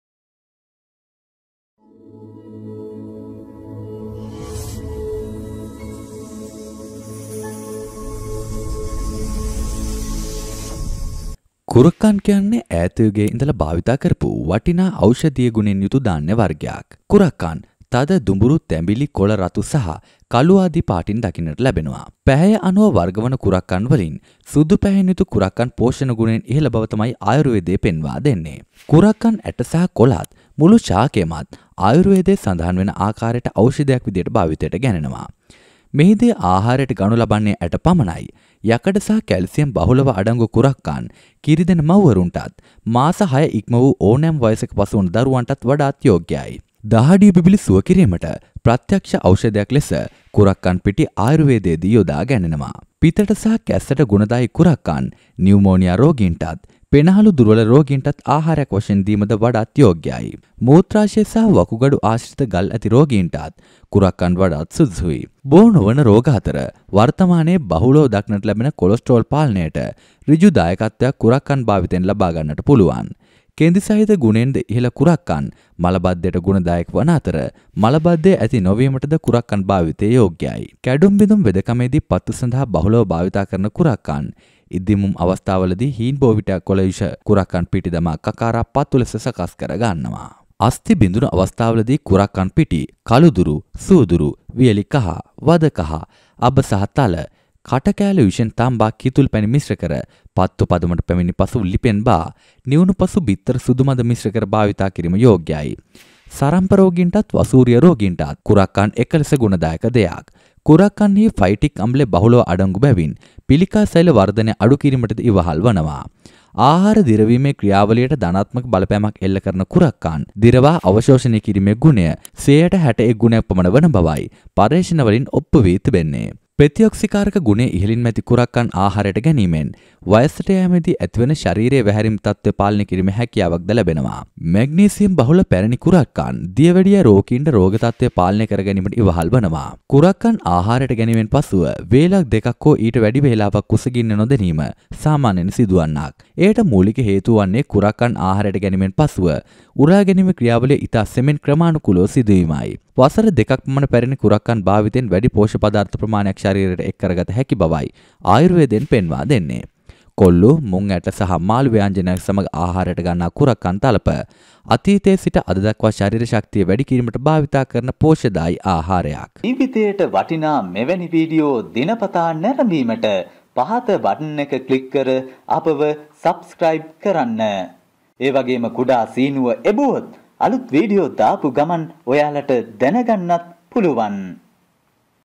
වකුගඩු ගල් සුව කරන කුරක්කන්
දහඩිය බිබිලි සුව කිරිමට ප්‍රත්‍යක්ෂ ඖෂධයක් ලෙස කුරක්කන් පිටි ආයුර්වේදයේදි යොදා ගැනේ.